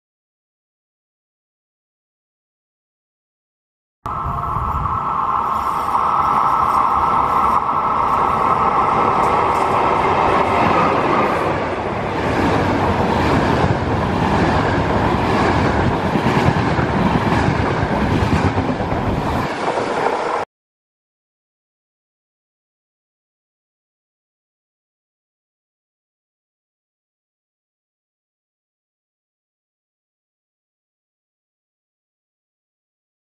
Treno intercity in transito a Firenze Castello il 12 Settembre 2021
Treno intercity con E401 in transito a Firenze Castello il 12 Settembre 2021